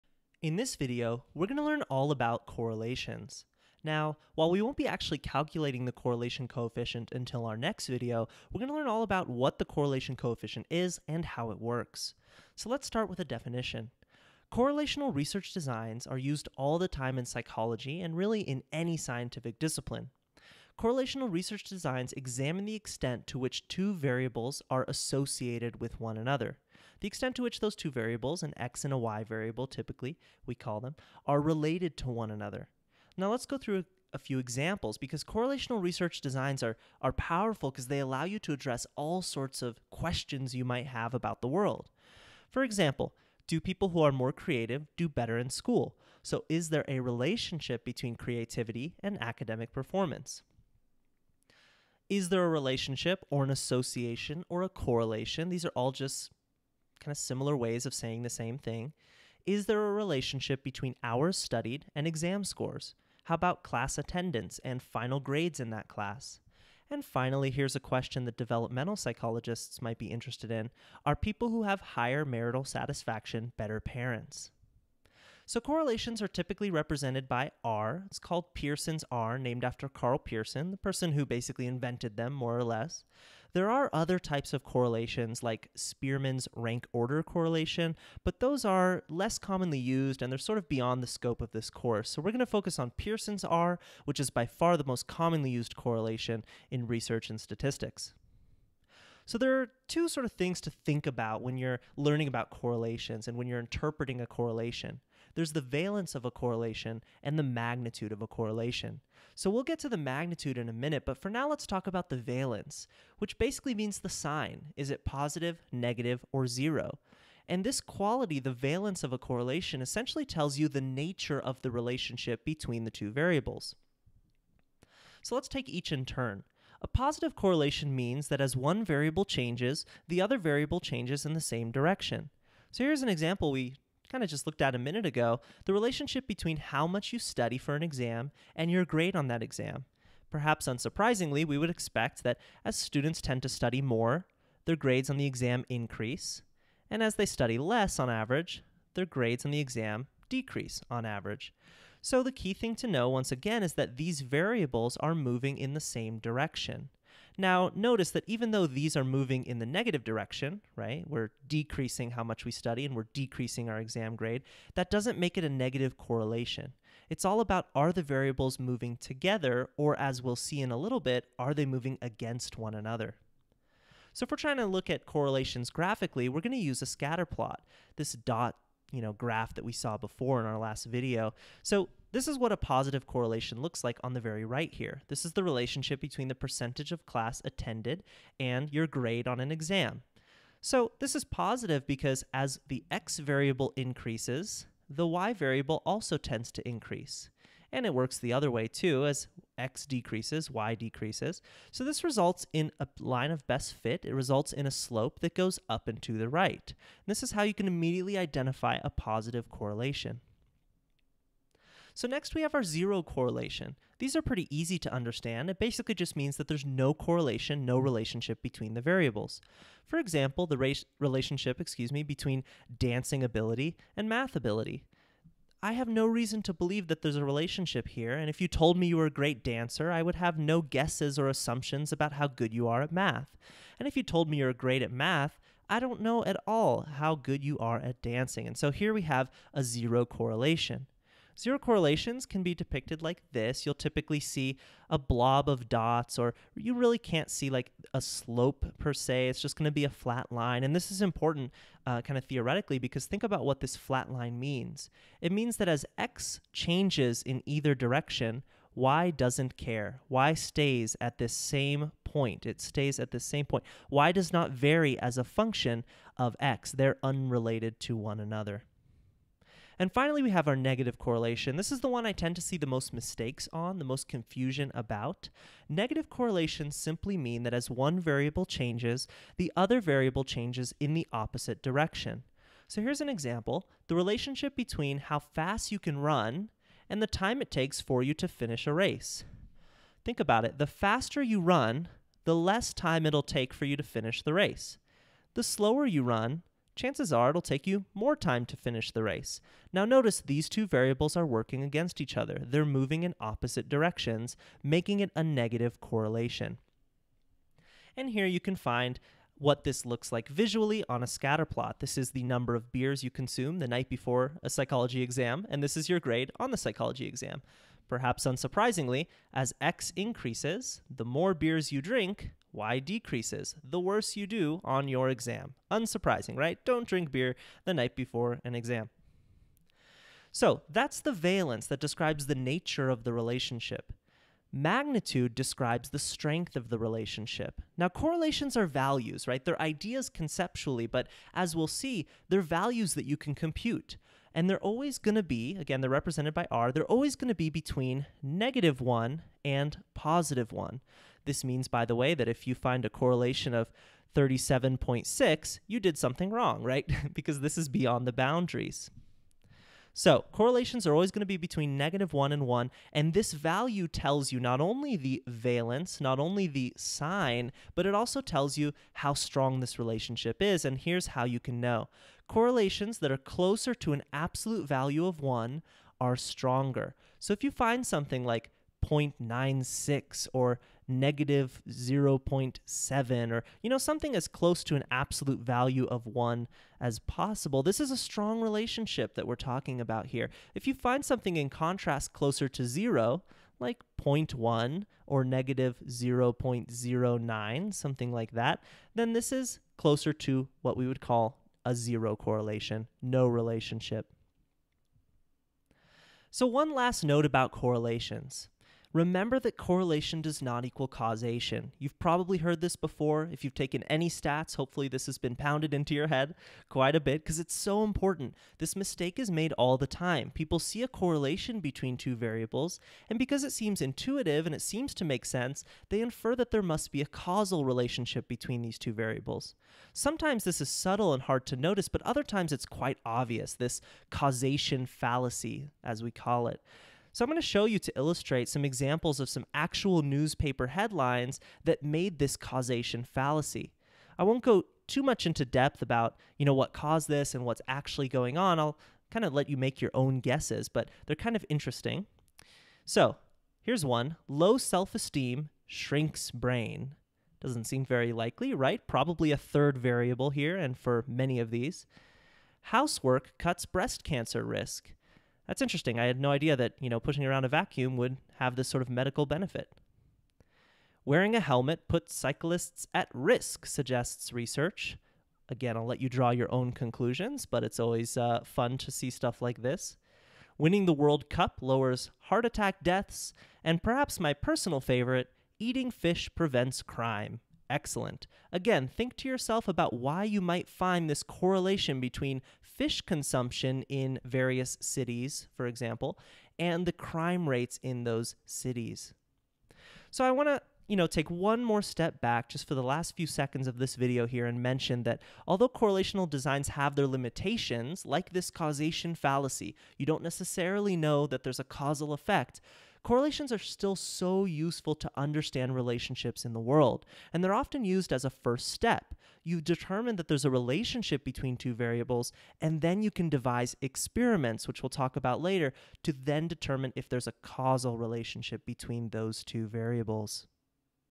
What Are Correlations?
Correlations are used far and wide to describe the relationship between two variables (typically referred to as an "x" variable and a "y" variable). In this video, we'll discuss how to understand and interpret a correlation coefficient, the value we compute to quantify this relationship.